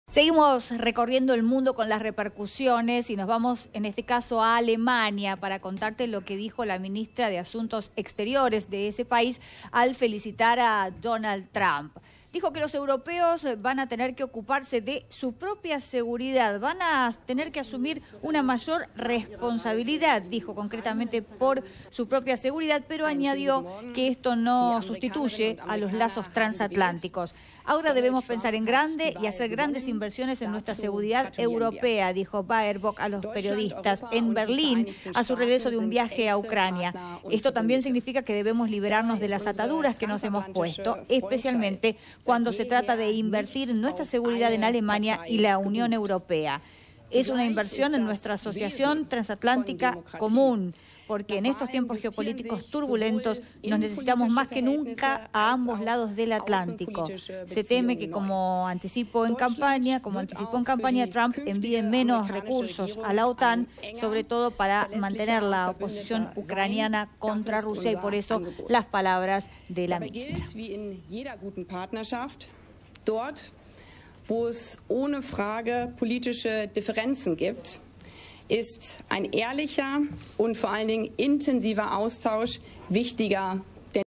ELECCIONES EN EEUU | ALEMANIA tras el triunfo de TRUMP
👉 "Los europeos deben asumir una mayor responsabilidad por su propia seguridad", dijo la ministra alemana de Asuntos Exteriores al felicitar a Donald Trump por su victoria en las elecciones presidenciales de Estados Unidos, pero añadió que esto no sustituye a los lazos transatlánticos.